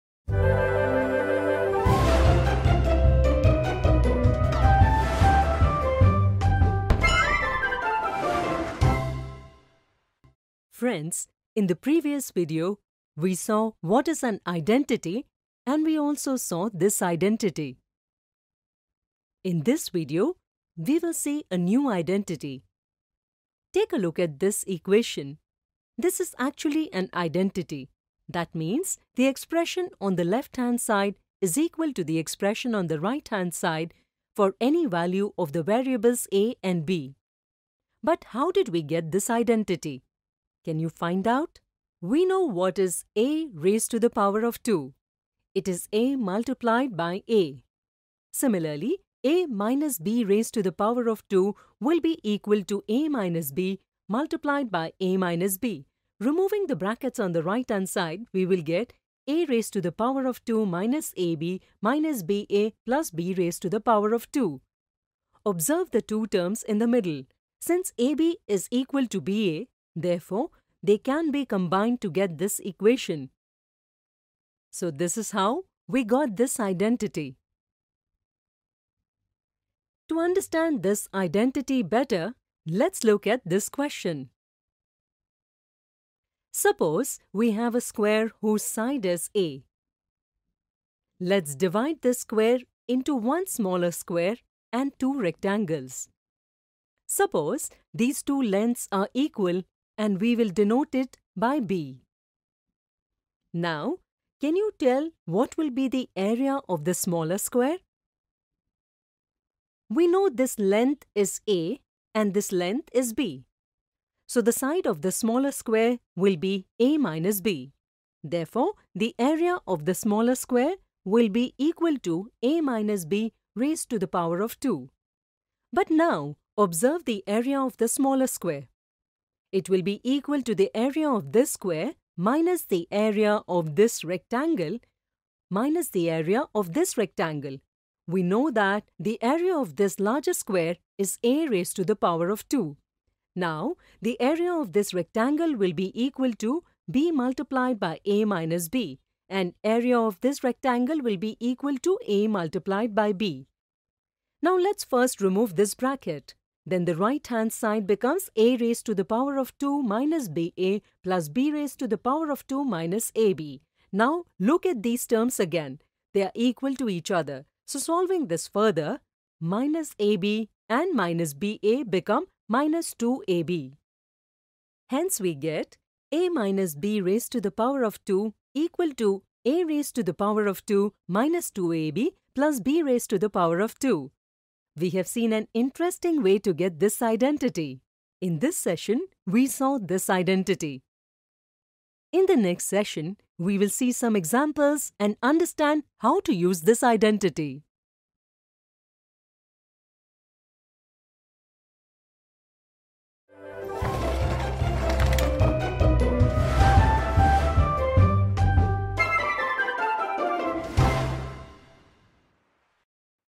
Algebraic Identity:(a-b)^2=a^2+b^2-2ab | Part 1/3 | English | Class 8
Algebraic Identity:(a-b)^2=a^2+b^2-2ab | Part 1/3 | English | Class 8 | NCERT

Algebraic Identity II | Algebra | Grade 8 | NCERT | Algebraic expressions and identities

Welcome to part 1 of Algebraic Identity II. In this video, we will discuss the concept of Algebraic Identity II.